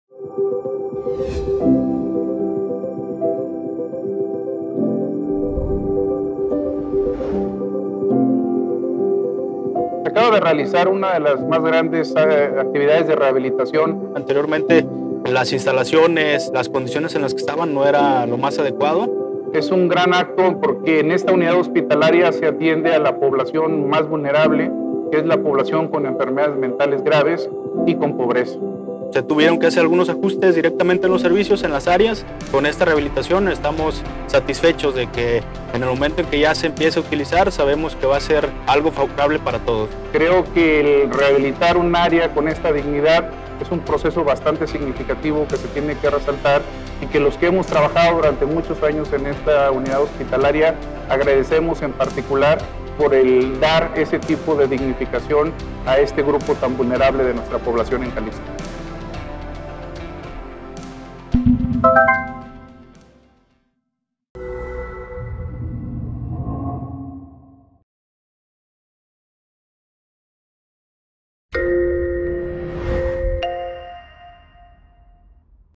El Zapote en rehabilitación
Basta decir que el hospital psiquiátrico ElZapote no se intervenía desde los 80 para que te des apenas una idea de las condiciones en las que se encontraba; era inhumano. Ahora mira lo que estamos haciendo para convertirlo en un espacio digno